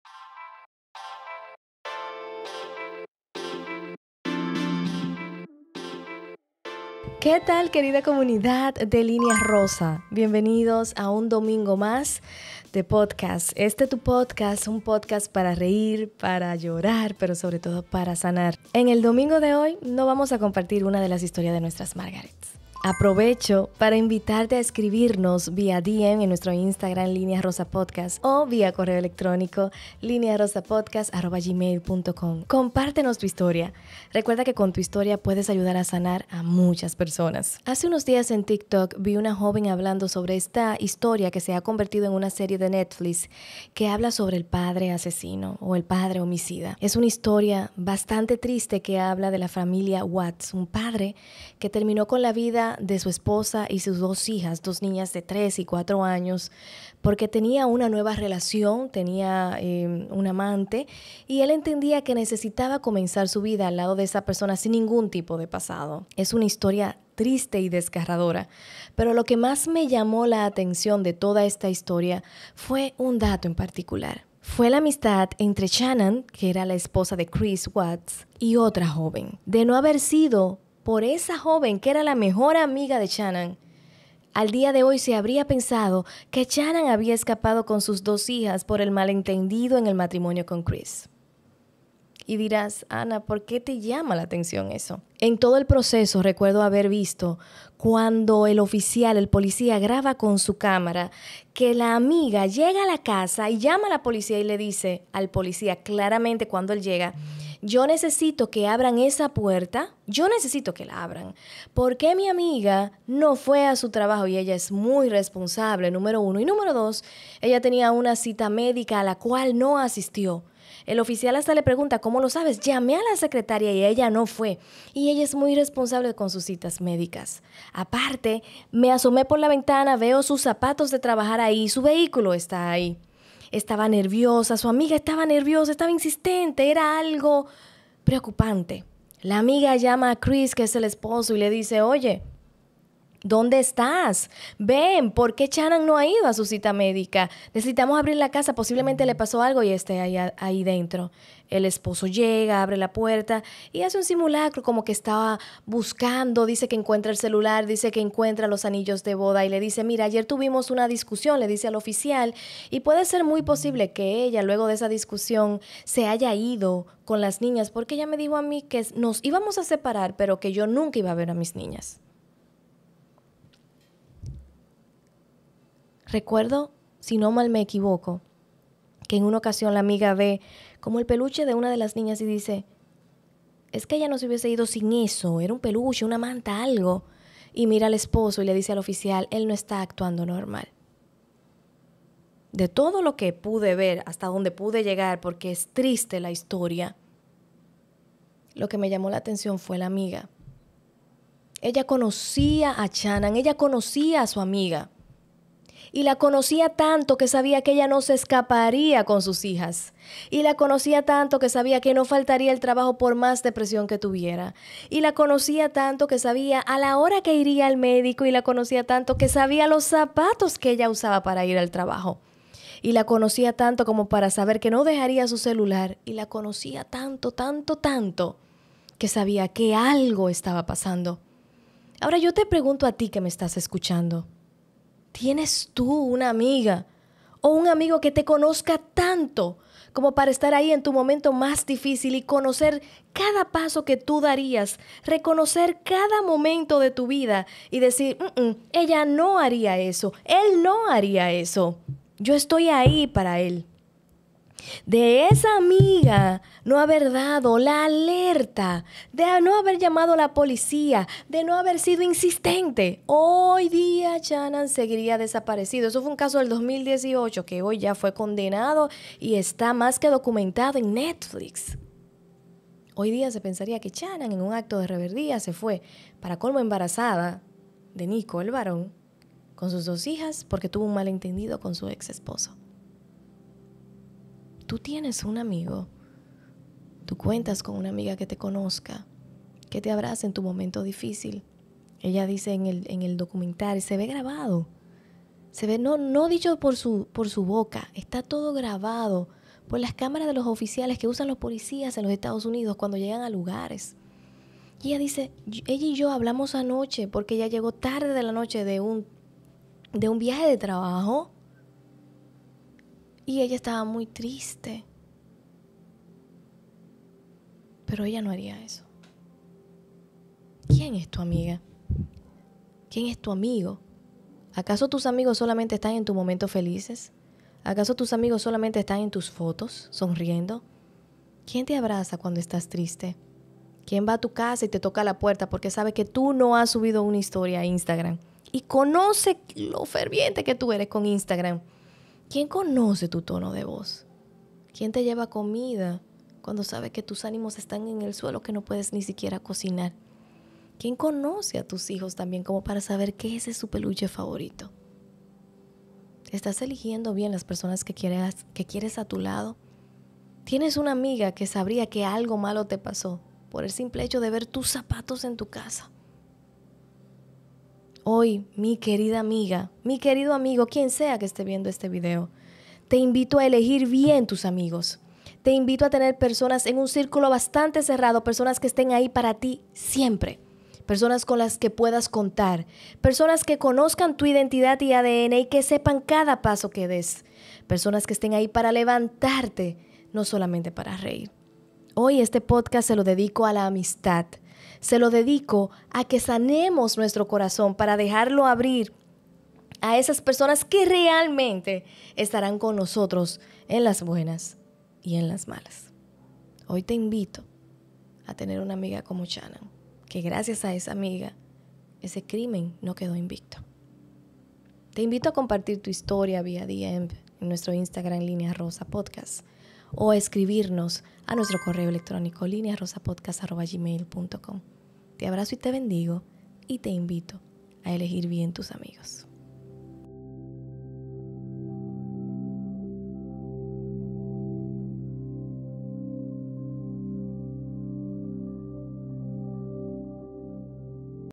💌 ¿QUIEN ES ESA AMIGA??! MORALEJA DE LA TRISTE HISTORIA DE LA FAMILIA WATTS LINEAROSAPODCAST ✨💖
Hace unos días tuve la oportunidad de ver parte de la historia de la familia Watts gracias al documental de netflix llamado el padre homicida, esta serie trata del recuento de los hechos de como un esposo acaba con la vida de su esposa embarazada y sus hijas porque entendía que necesitaba tener un nuevo comienzo al lado de su nueva pareja, toda esta triste historia huno algo que impacto mi corazón y fue como gracias al amor de una amiga este caso no quedo invicto, de esta amiga no haber dado la voz de alerta , de no haber identificado irregularidades basado en el conocimiento de su amistad este caso aun seguiría siendo un gran misterio. Esto me trajo varias peguntas a mi mente y sobre todo a mi corazón.
 Me pregunte quien sería mi amiga, esa que identificaría mis actitudes y que sabría identificar, defenderme y decir , ella no haría o haría esto. Hoy te animo a elegir ese amigo a identificarlo y a crear amistades saludables que estén contigo en las buenas y en las malas.